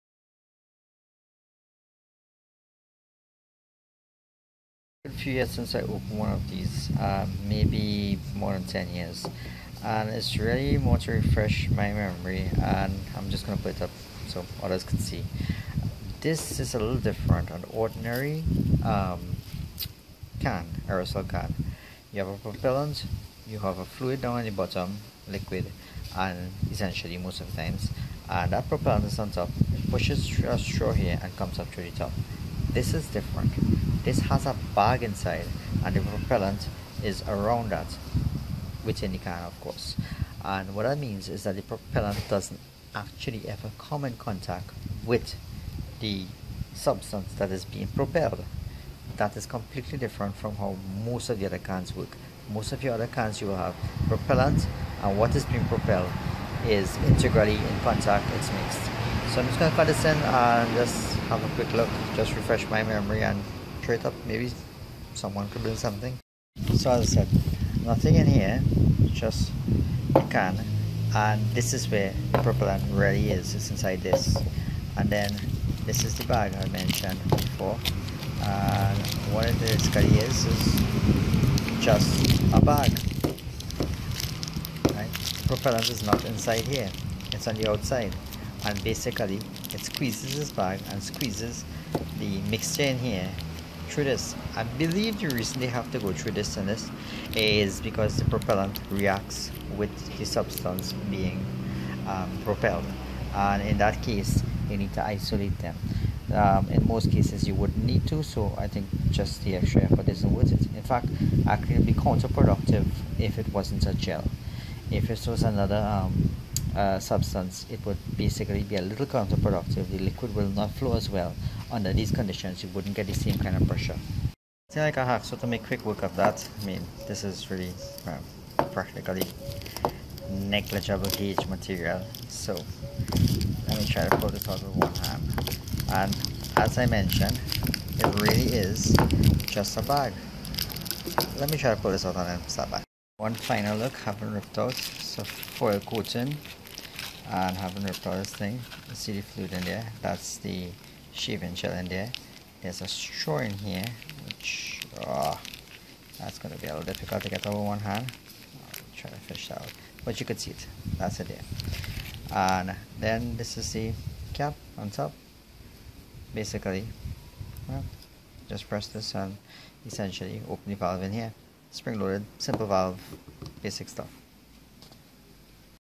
040 Shaving Gel Can Disassembly
Quick disassembly of a shaving gel spray can to show the different propellant and working fluid configuration used.